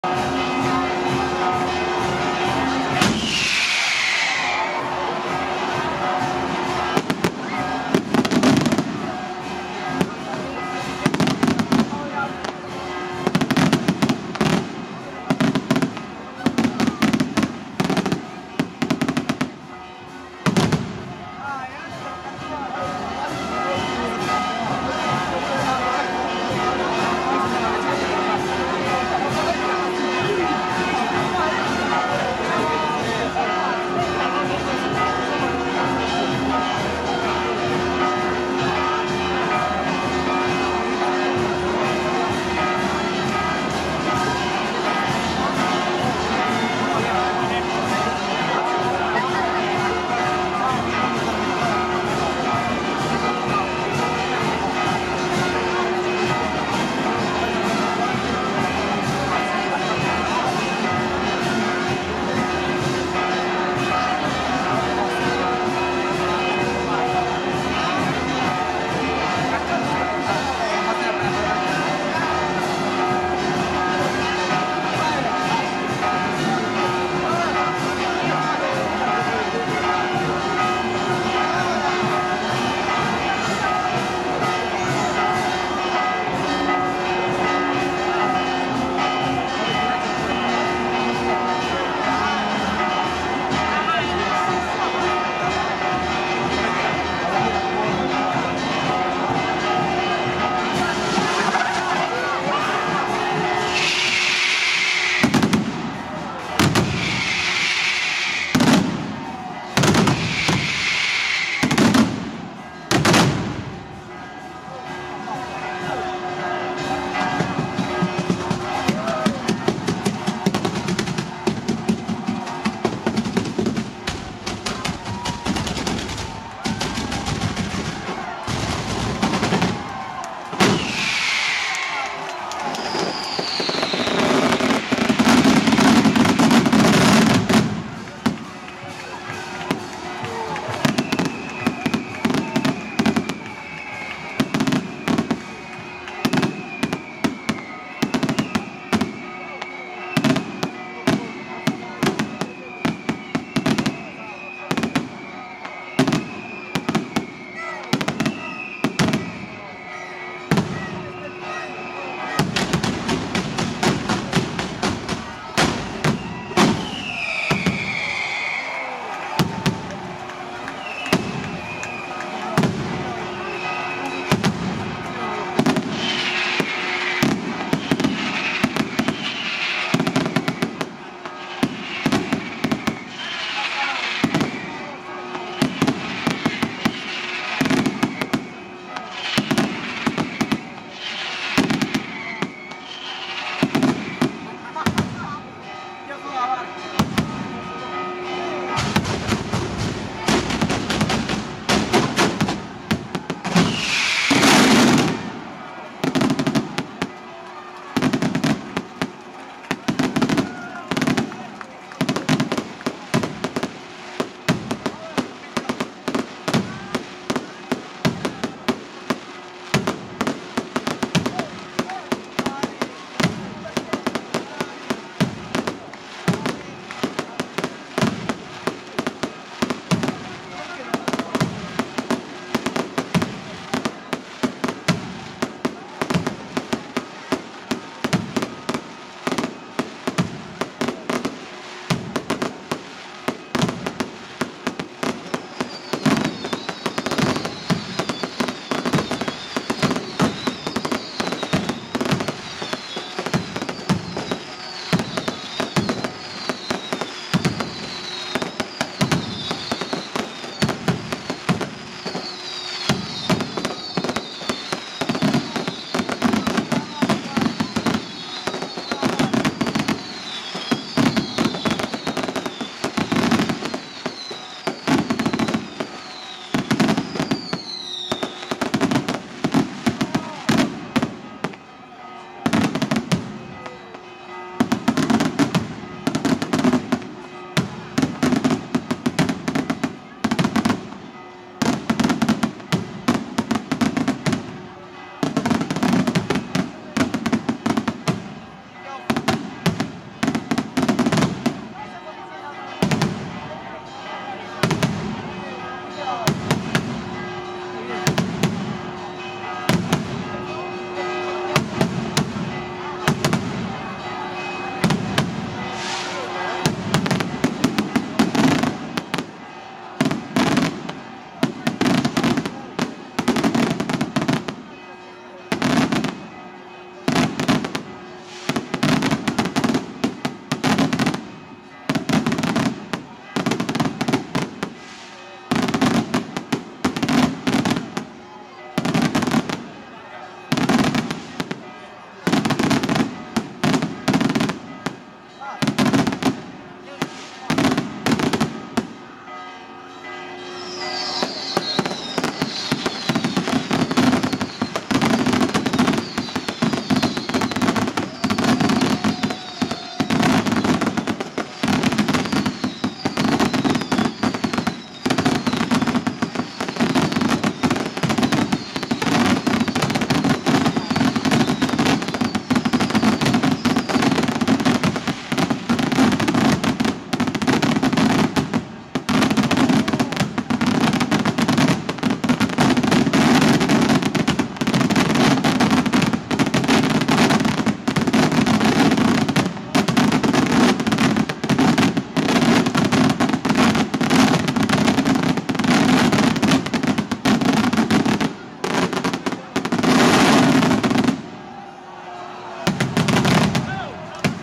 Palazzolo 10 Agosto 2022 Spettacolare SCIUTA di San Sebastiano e Spettacolo Pirotecnico FIRE SUD 4K
Solenni Festeggiamenti in Onore di San Sebastiano Martire.
Glorioso Protettore della Città di Palazzolo Acreide (SR).
Uscita dalla Monumentale Basilica della Processione della Reliquia.
E Spettacolare "SCIUTA" dell'Artistico Simulacro di San Sebastiano Martire.
Tradizionale lancio di "n'zareddi" e di carta colorata e Spettacolo Pirotecnico Diurno realizzato dalla Rinomata e Prestigiosa Azienda Italiana: "FIRE SUD" da Zafferana Etnea (CT).
Piazza del Popolo di Palazzolo Acreide (SR), Mercoledì 10 Agosto 2022 - Ore 13:00.
Buona Visione!!!